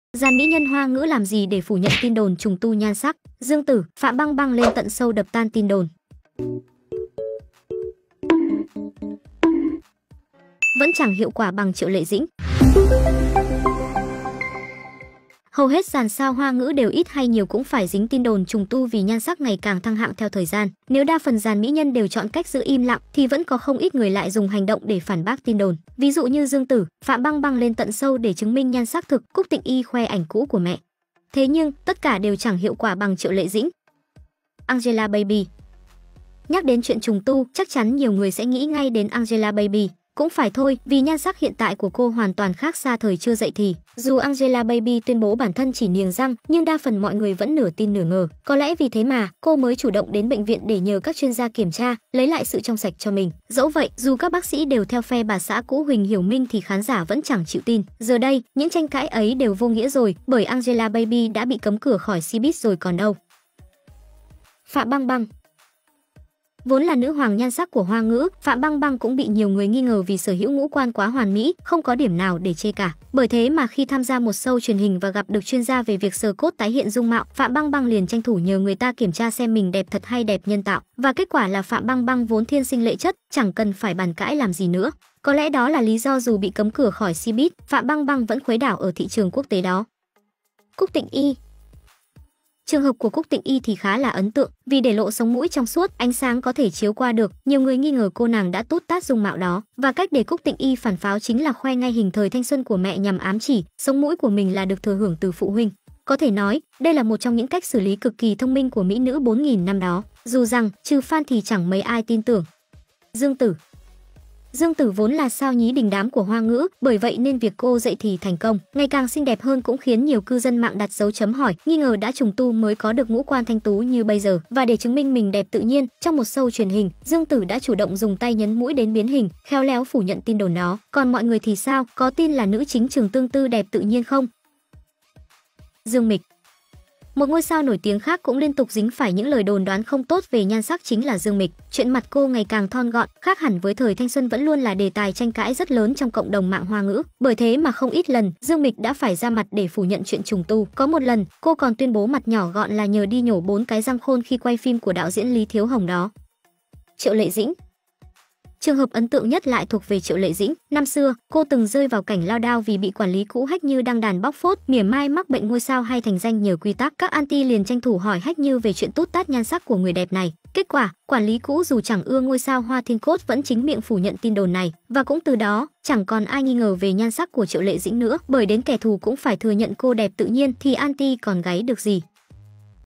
Dàn mỹ nhân phủ nhận tin đồn "trùng tu" nhan sắc: Dương Tử - Phạm Băng Băng lên tận show
Hầu hết dàn sao Hoa ngữ đều ít hay nhiều cũng phải dính tin đồn "trùng tu" vì nhan sắc ngày càng thăng hạng theo thời gian. Nếu đa phần dàn mỹ nhân đều chọn cách giữ im lặng thì vẫn có không ít người lại dùng hành động để phản bác tin đồn. Ví dụ như Dương Tử - Phạm Băng Băng lên tận show để chứng minh nhan sắc thực, Cúc Tịnh Y khoe ảnh cũ của mẹ. Thế nhưng, tất cả đều chẳng hiệu quả bằng Triệu Lệ Dĩnh.